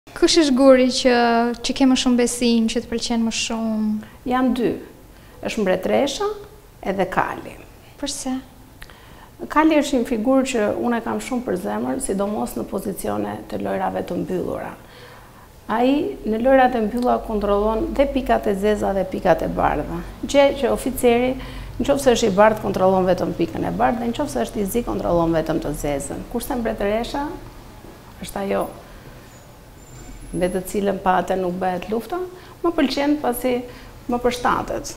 PARFUME - Kush eshtë guri më i fortë për kampionen e shahut?
Kali dhe Torra, jane dy gurët e fitores për kampionen Egla Shabanaj. Përse kështu?!